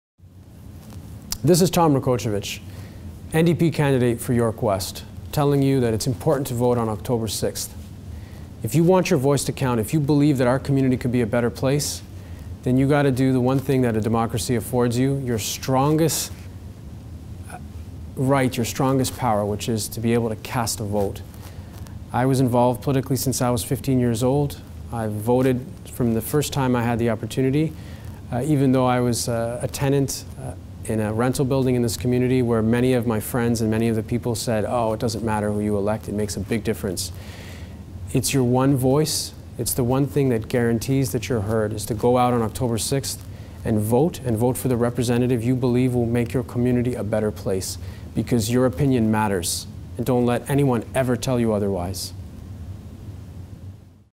2011 Election Message From Tom Rakocevic
A message from NDP MPP Candidate, Tom Rakocevic.